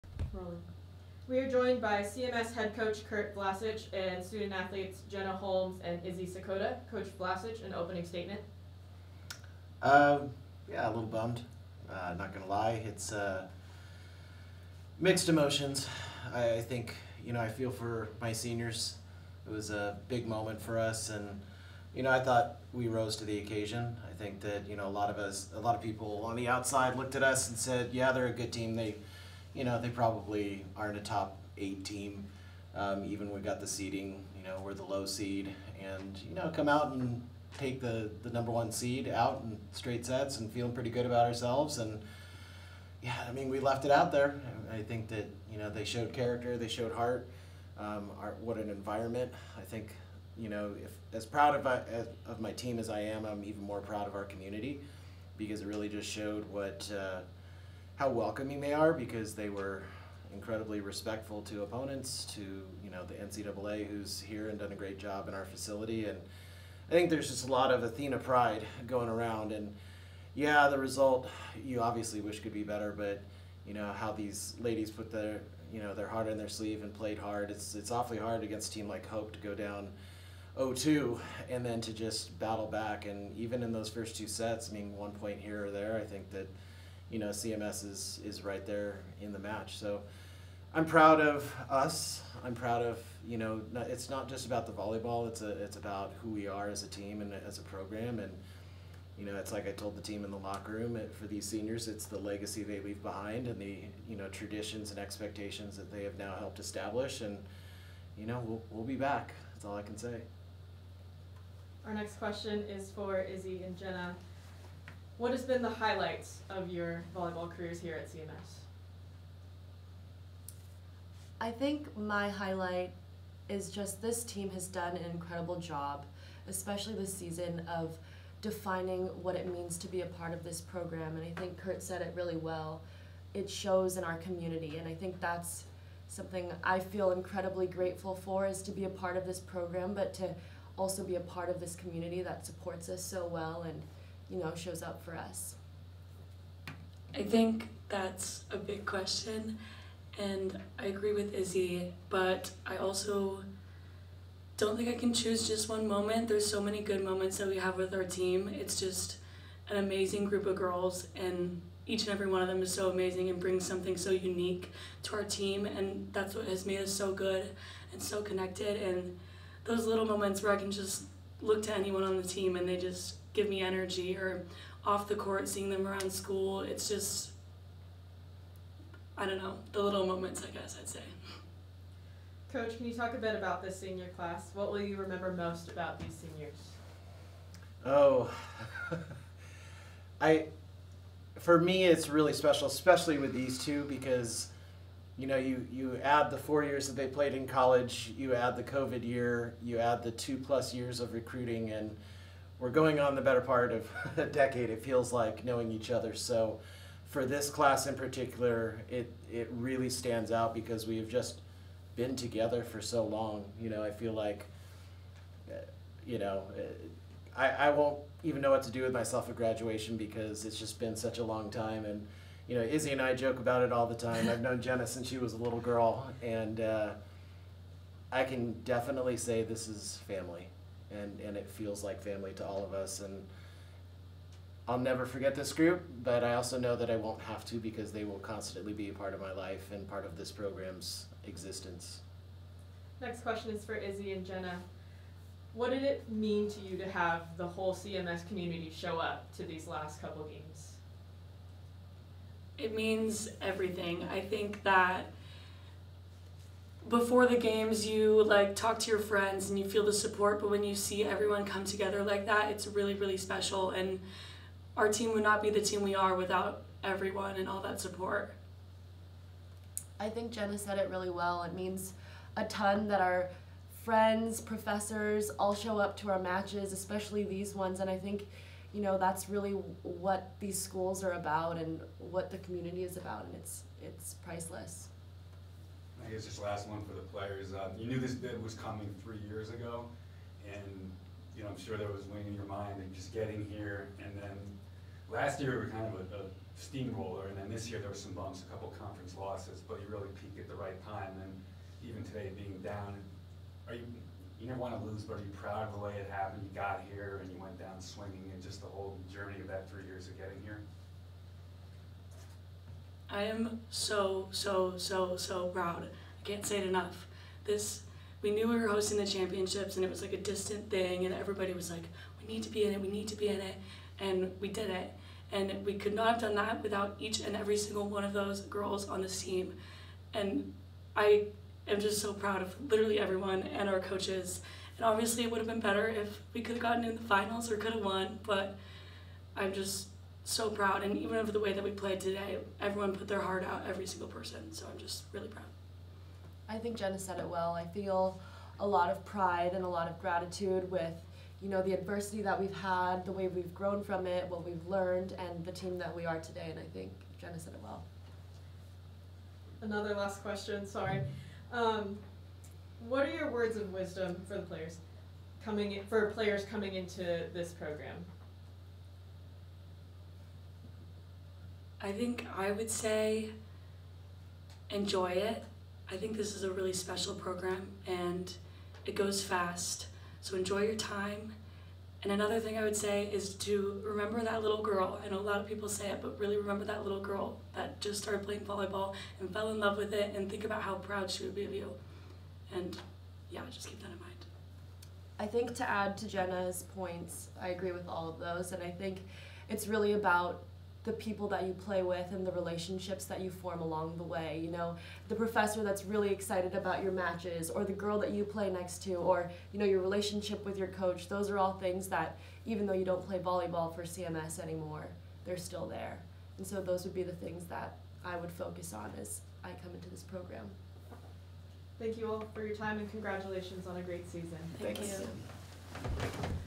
CMS Press Conference - NCAA Division III Semifinals
The CMS women's volleyball team kept fighting back to force the match to a full five sets after falling behind 2-0 but came up short to Hope in the NCAA Division III Semifinals. Hear from the Athenas after their valiant comeback in front of a loud home crowd ended in a tough, but proud defeat.